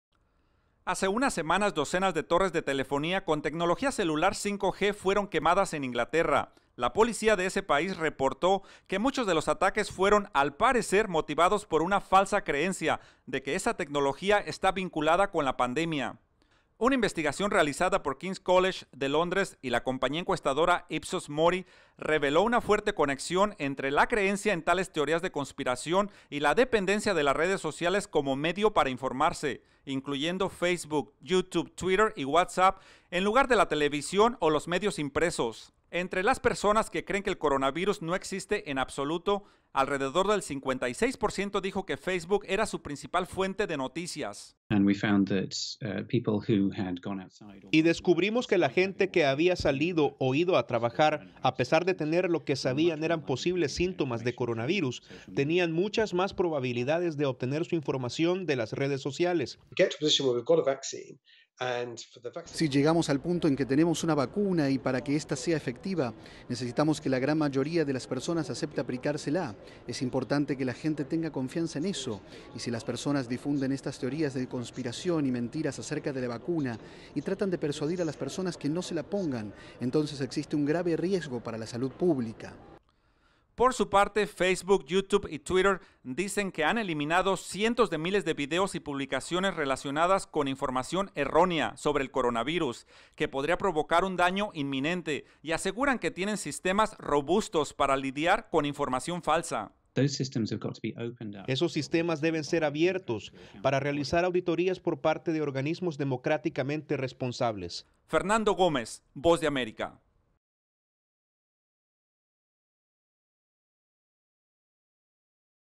Estudios revelan que usuarios de redes tienen más probabilidades de creer en teorías conspirativas
La pandemia del coronavirus ha inspirado todo tipo de teorías de conspiración, alrededor del mundo. Aparentemente son también quienes más ignoran las reglas de confinamiento, según revelo una nueva investigación. Desde Washington, Fernando Gomez amplía esta información.

¡Visítenos!